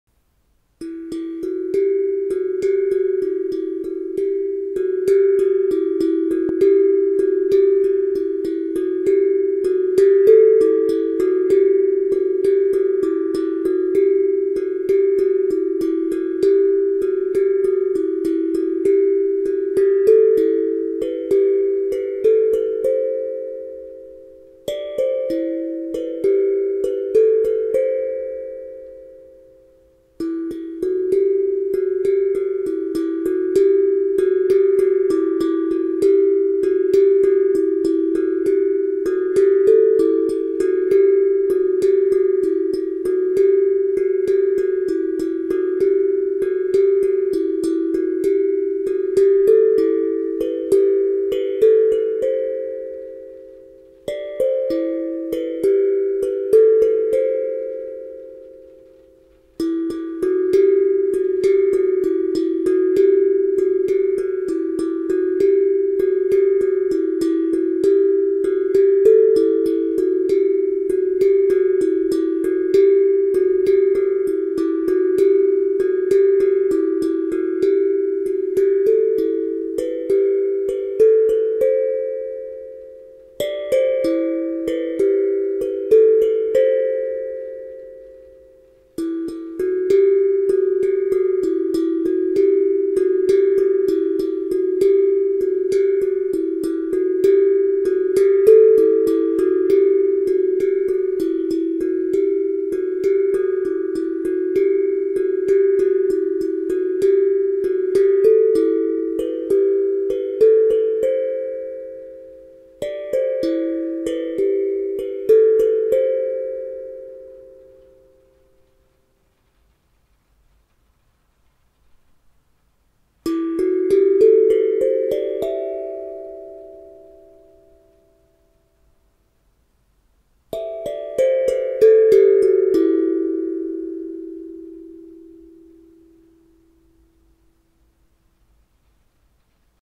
T drum △ tank drum ▽ stainless steel ø 22cm △ e minor
E4 F# G A B C D E5_440 Hz
Model jednostronny/ One sided model
Góra stal nierdzewna (języki), dół stal węglowa, średnica 22cm/
Stainless steel top (tongues), carbon steel bottom, diameter 22cm
Malowanie proszkowe_transparent jasny niebieski/Powder coating_transparent light blue